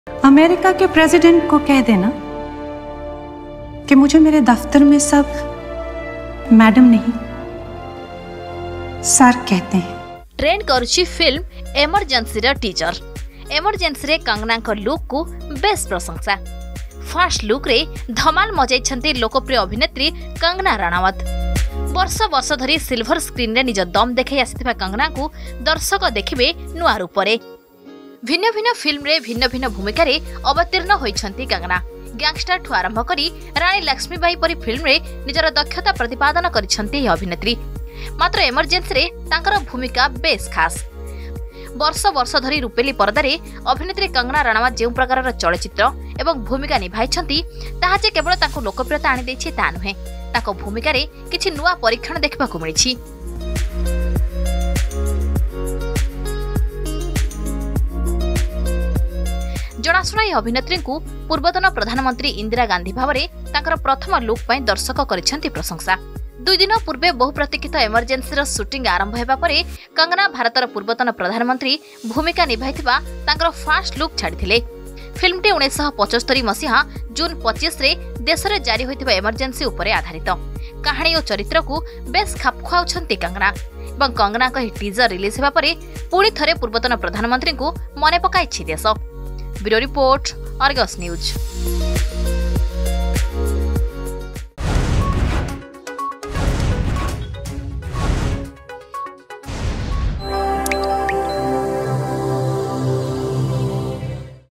First Look Of Movie 'Emergency' is Trending
First Look Of Movie 'Emergency' is Trending , Kangana Ranaut is nailing with her looks and acting. Former Prime Minister Indira Gandhi's Look is been recreated by the actress in the movie.